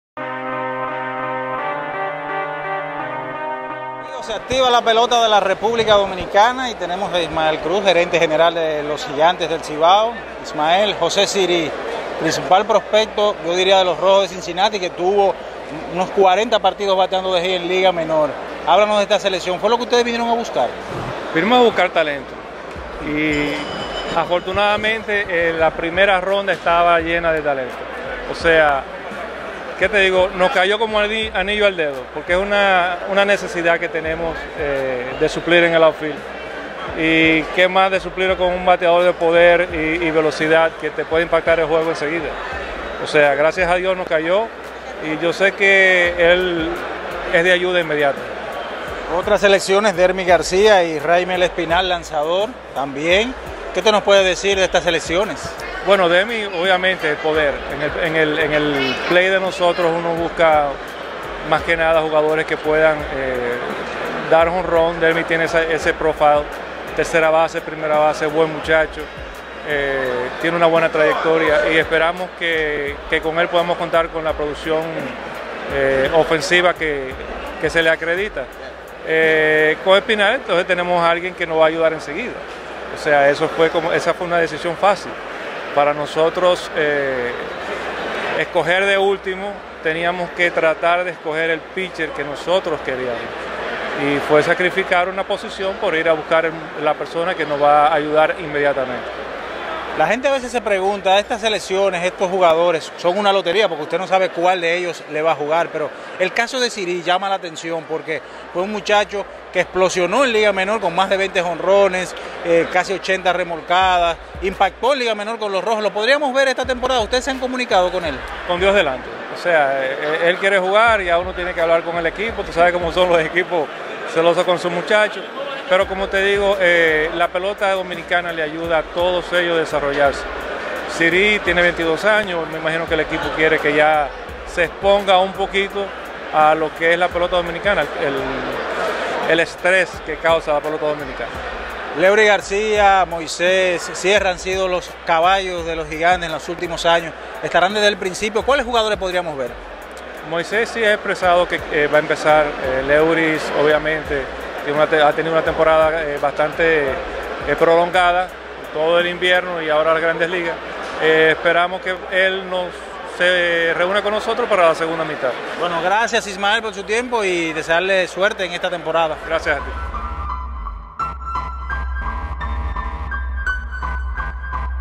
Ismael Cruz Gigantes del Cibao
Entrevista con Carlos José Lugo gerente de operaciones de los Tigres del Licey luego del Draft LIDOM 2017.